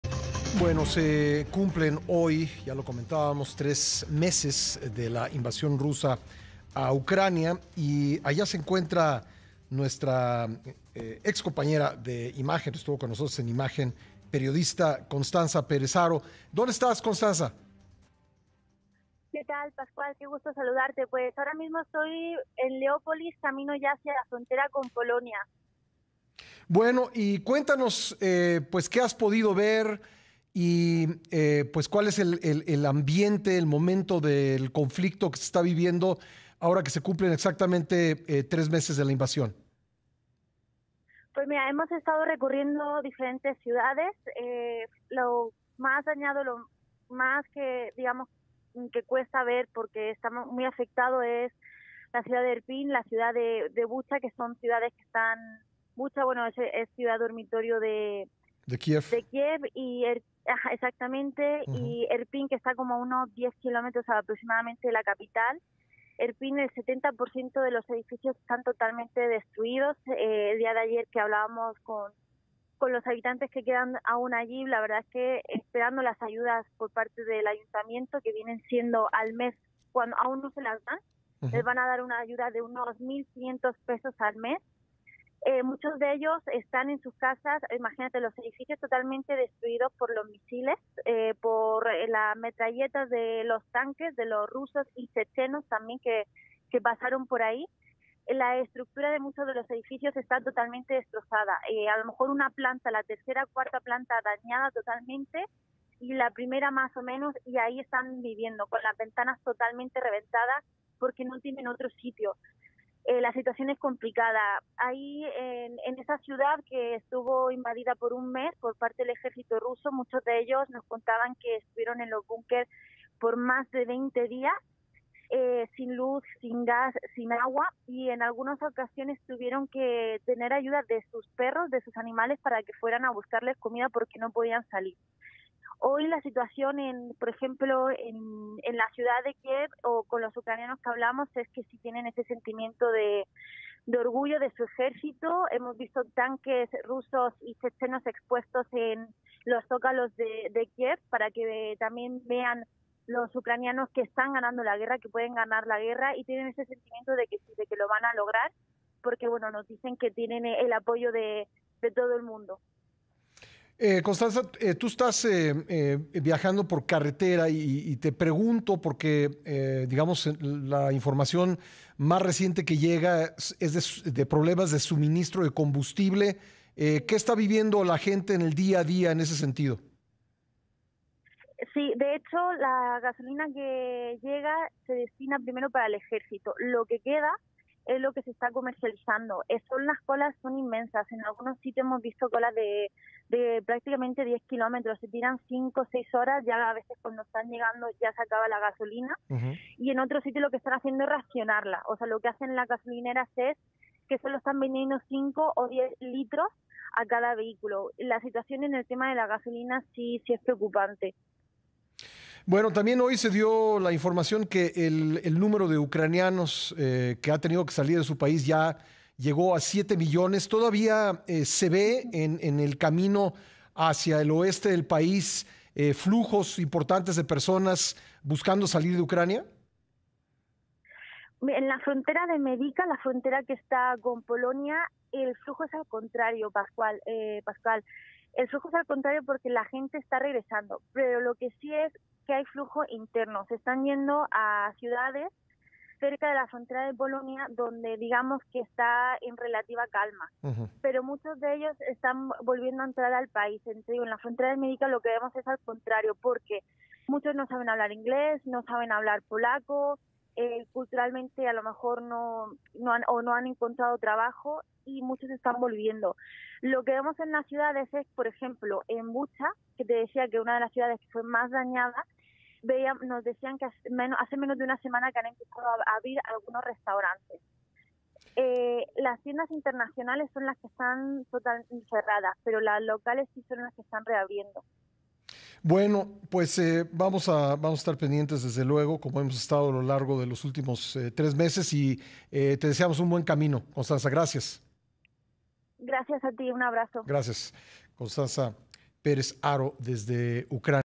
A tres meses de la invasión rusa a Ucrania
Luego de tres meses de la invasión rusa a Ucrania se puede notar la destrucción de edificios y de las familias que siguen tratando de aferrarse a sus hogares, hay escasez de gasolina y algunos alimentos que no pueden cruzar la frontera.